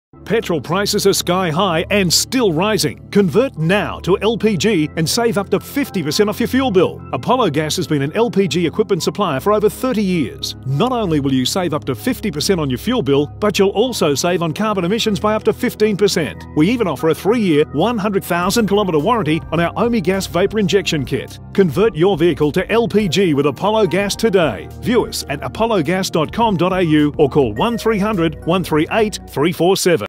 Apollo GAS Products 30s TVC
30s TVC for Apollo Gas Products, a supplier of quality LPG conversion kits for your vehicle.